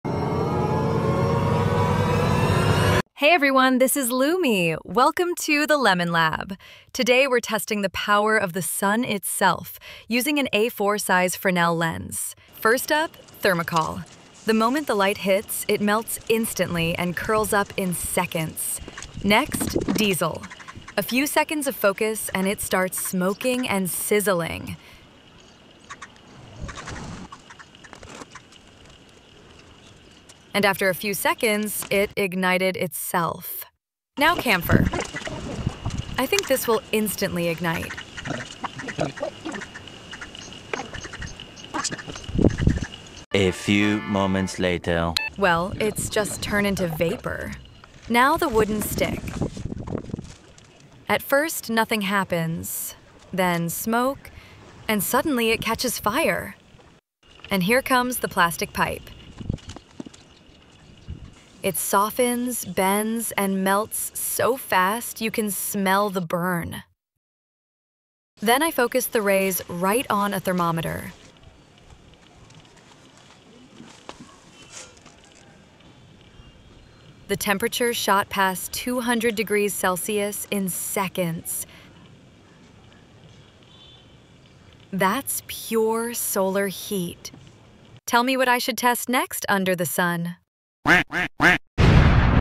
What Happens When a Fresnel Lens Focuses on Diesel?
🔥 Fresnel Lens Burn Test 🔥
Watch as I focus sunlight using a Fresnel lens to burn and melt everyday materials — diesel, thermocol (styrofoam), and plastic pipe. The power of concentrated sunlight is insane! ☀️⚡

⚠️ Warning: This experiment is for demonstration only.
Do NOT try this at home. Burning plastics and fuels releases toxic fumes and can be dangerous. Always perform such tests outdoors, away from flammable materials, with proper safety gear and supervision.